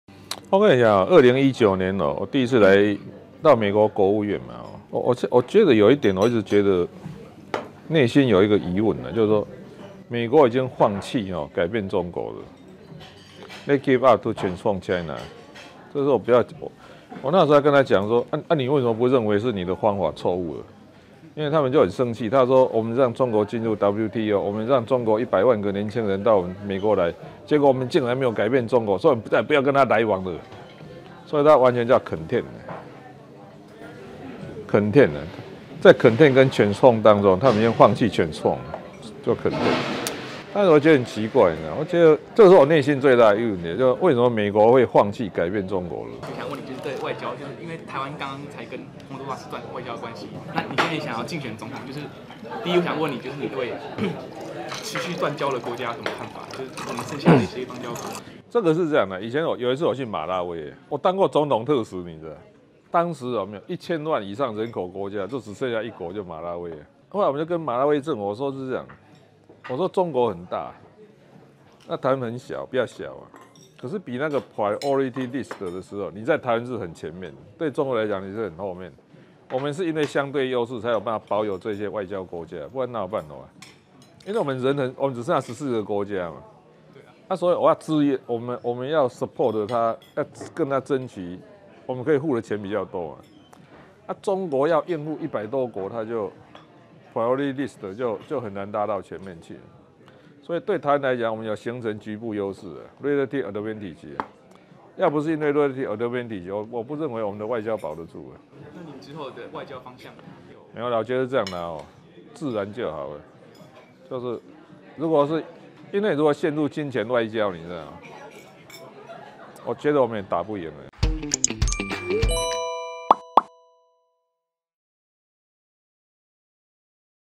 宏都拉斯斷交陷邦交危機？柯文哲提台灣外交「這點」是優勢【CNEWS】
CNEWS匯流新聞網記者 陳儀潔 陳巧紜／台北報導